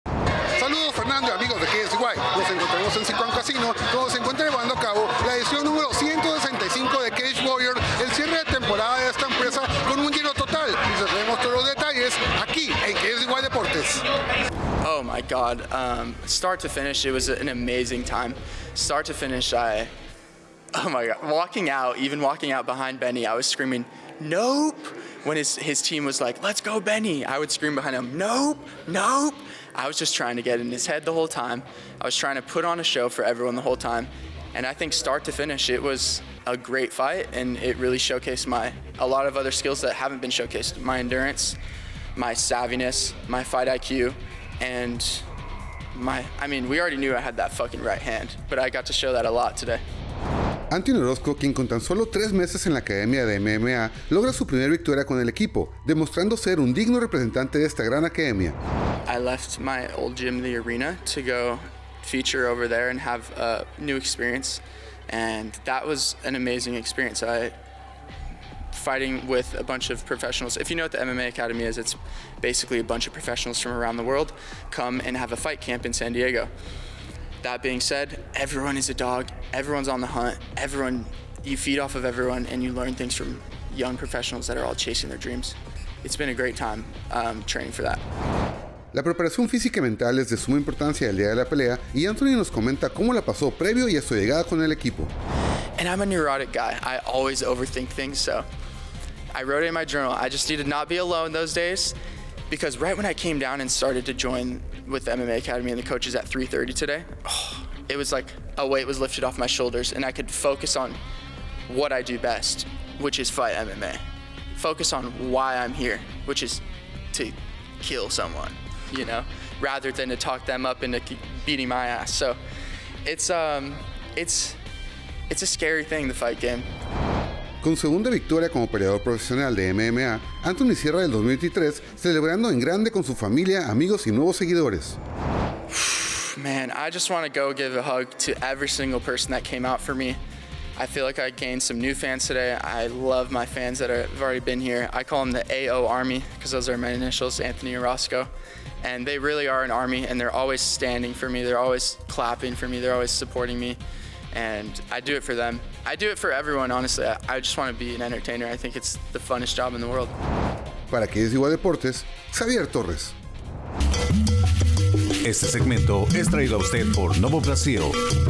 Anthony Orozco logra victoria en Cage Warriors 165
Anthony Orozco se lleva la victoria en Cage Warriors #165 al 3er round en Sycuan Casino Resort.